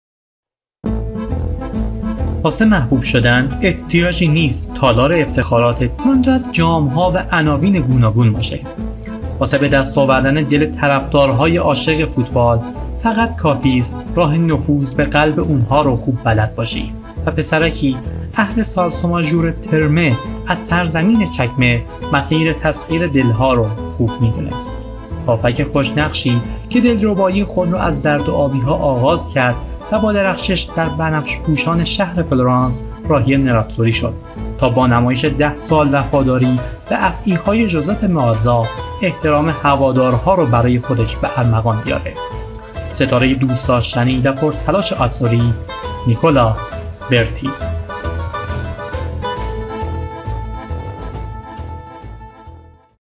Nicola Berti - نیکولا برتی
Nicola Berti
Born in 14 April 1967
He is an Italian former footballer, who played as a midfielder. Berti's career spanned three decades, during which he played for several clubs: after beginning his career with Parma, he played with Fiorentina, and in particular Inter Milan, where he became an important figure in the club's midfield, winning a Serie A title and three UEFA Cups. After his time in Italy, he ended his career with spells in England, Spain and Australia, at Tottenham, Alavés, and Northern Spirit respectively.

نیکولا برتی
زاده 14 آپریل 1967
هافبک سابق تیم ملی ایتالیا ، که همراه با آتزوری عنوان سومی جام جهانی 1990 و نایب قهرمانی جام جهانی 1994 را تجربه کرده است . او فوتبال را از باشگاه پارما آغاز کرد و پس از بازی در فیورنتینا راهی اینترمیلان شد و با این تیم قهرمانی در سری آ ، جام یوفا و سوپر کاپ ایتالیا را به تالار افتخارات خود افزود ، او سابقه بازی در تاتنهام انگلیس را نیز در کارنامه دارد .

Narrator & Written : Shahram Erfanian
Edit : Saeide Saatchi & Shahram Erfanian
Collector : Saeed Monfared
نویسنده و راوی : شهرام عرفانیان
ادیت : سعیده ساعتچی - شهرام عرفانیان
گردآوری : سعید منفرد